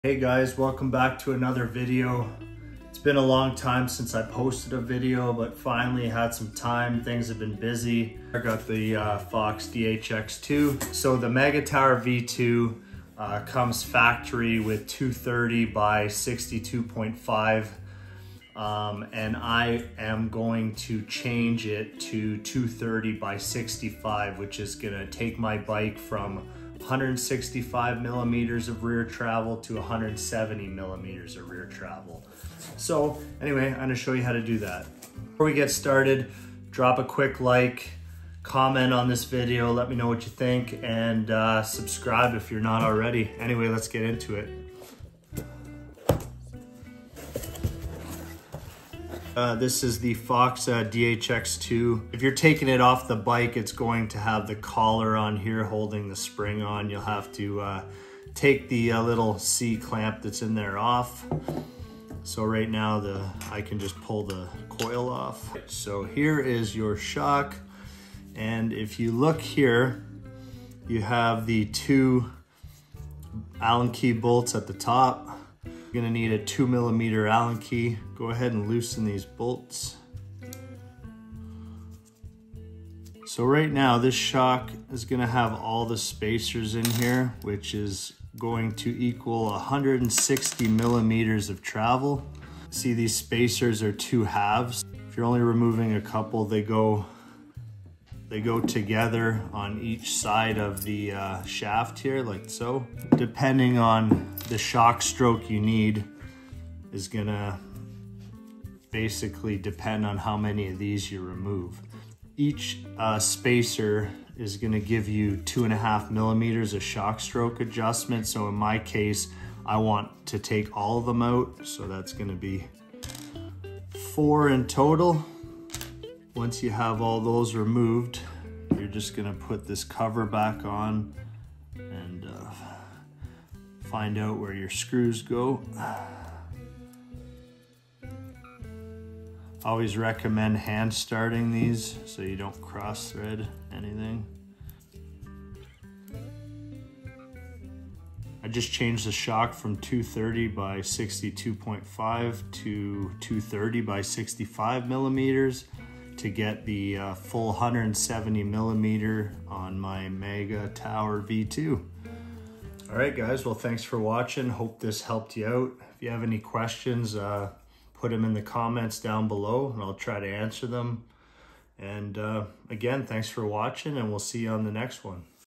CHANGE the shock stroke/travel on the new FOX DHX2 - EASY TUTORIAL
How to change the shock stroke on the new fox DHX2 . In my case i'm going to change the travel from 165mm to 170mm on my Santa Cruz Megatower V2. Got a great deal on a 230x60 shock but needed to make it 230x65 giving me the 170mm. This video will show you how to do it yourself .